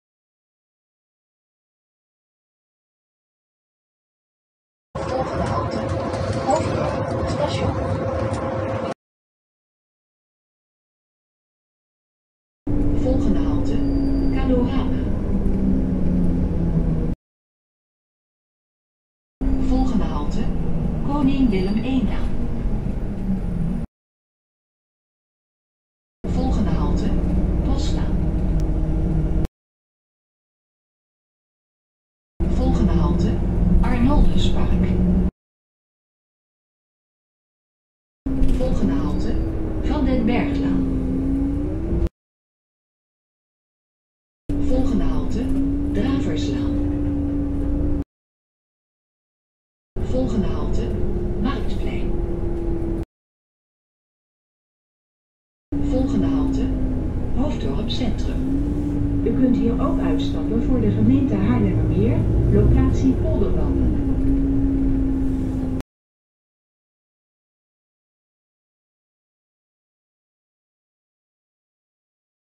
Omroepen Lijn 160 Connexxion Amstelland-Meerlanden: Hoofddorp Station - Hoofddorp Centrum
De omroepen en het Connexxion logo zijn eigendom van Transdev Nederland, deze video is enkel bestemd voor privégebruik en niet geschikt voor commerciële doeleinden.